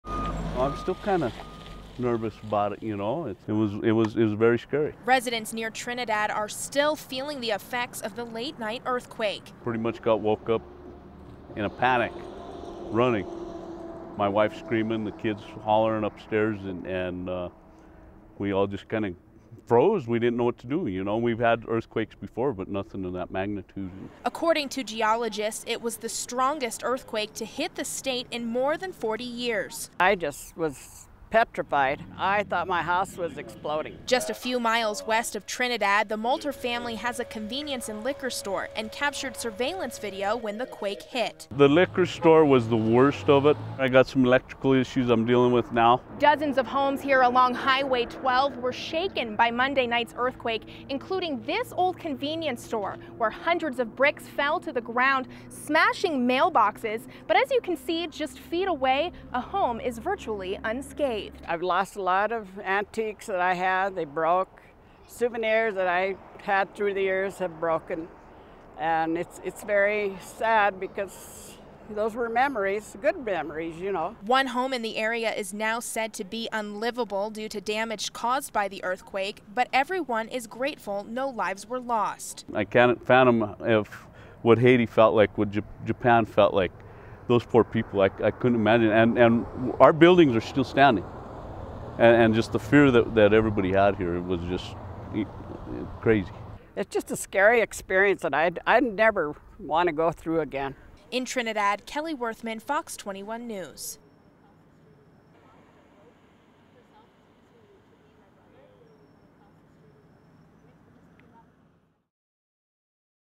Colorado earthquake
A 5.3 magnitude earthquake struck southern Colorado late Monday night.